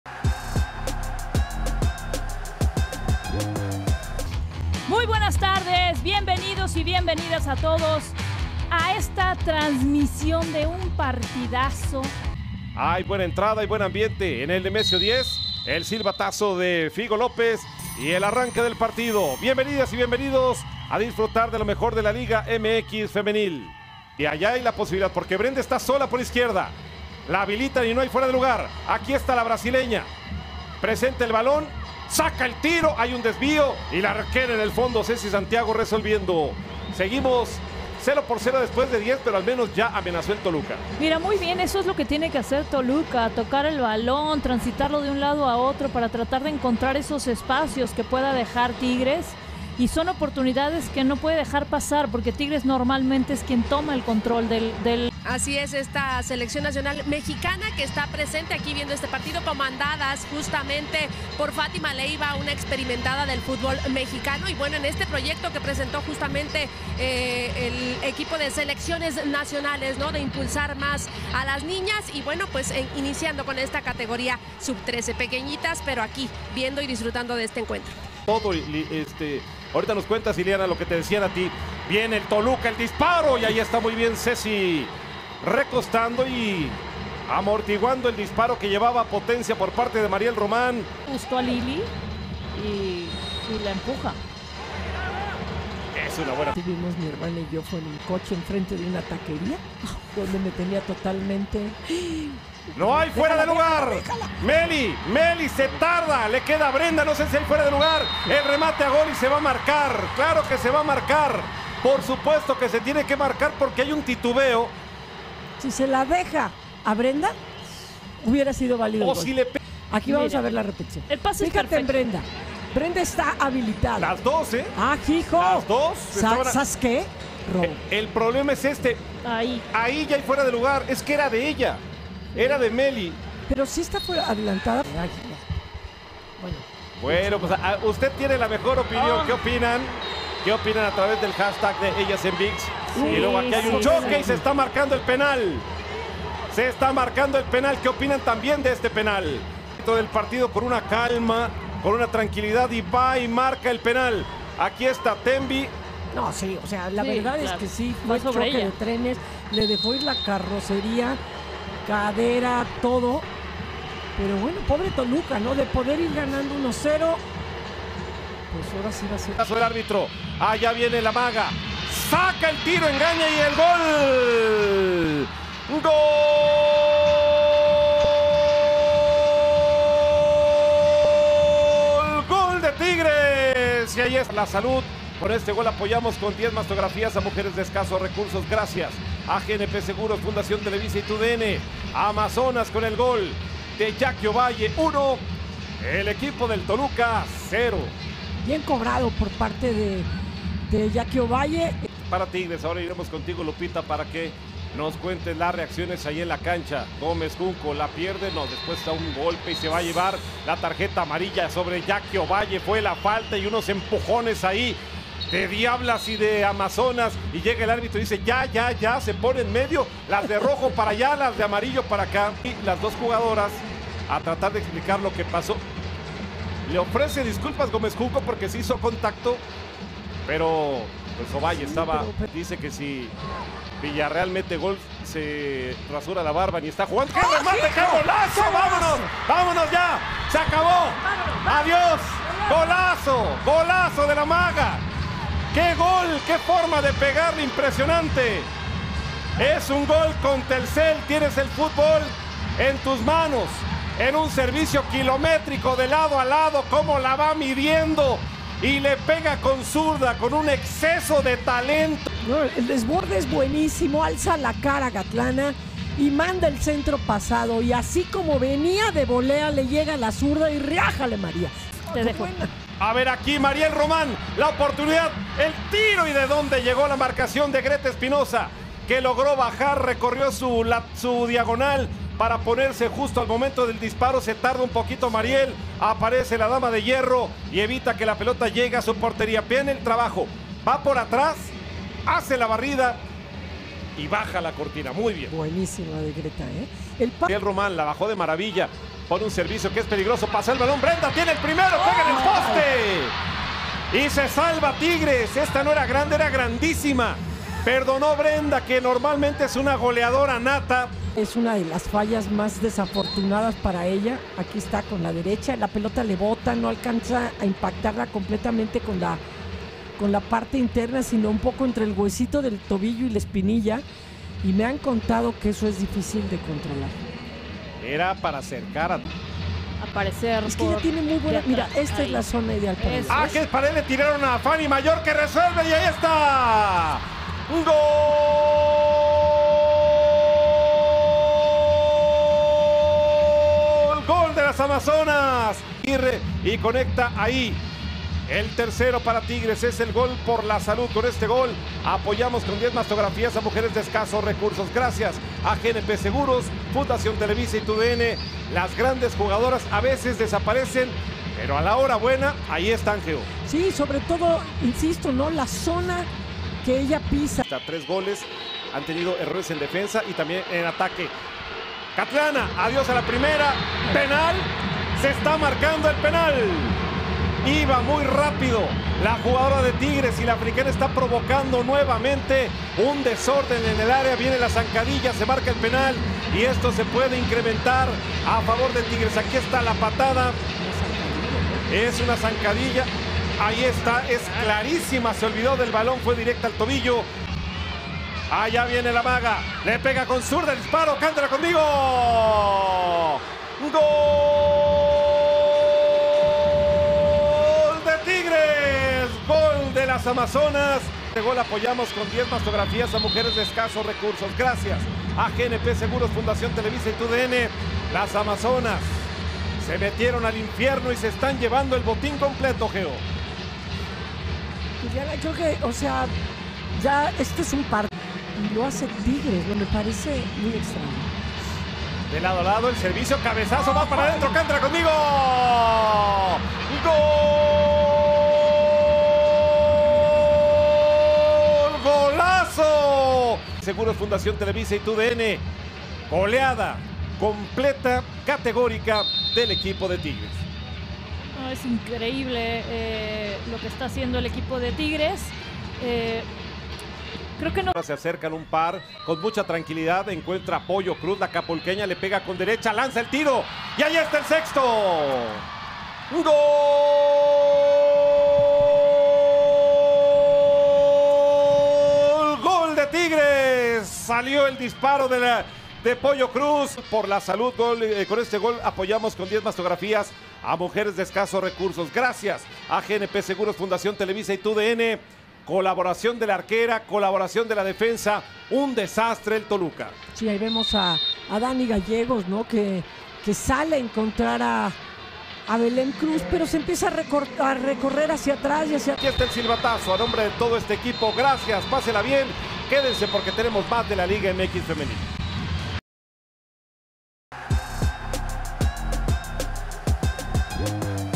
Resumen y goles | Toluca 0-6 Tigres | Liga Mx Femenil AP2024-J5 | TUDN
Las regias no tuvieron piedad y aprovecharon los errores en defensa de las Diablas para llevarse 3 puntos con media docena de goles a favor.